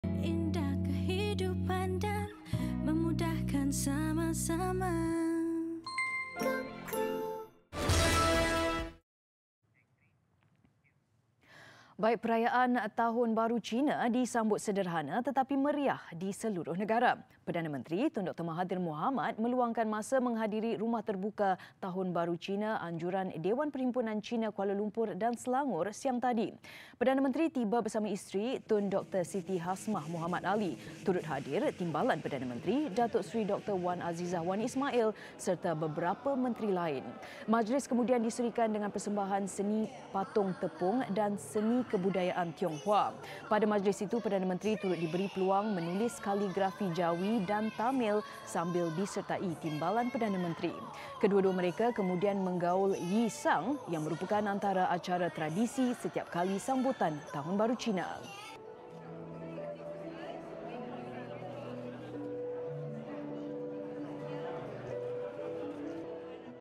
TAHUN BARU CINA | PM, TPM Kunjungi Rumah Terbuka
Perayaan Tahun Baharu Cina disambut sederhana tetapi meriah di seluruh negara.

Perdana Menteri, Tun Dr Mahathir Mohamad meluangkan masa menghadiri rumah terbuka Tahun Baharu Cina anjuran Dewan Perhimpunan Cina Kuala Lumpur dan Selangor siang tadi.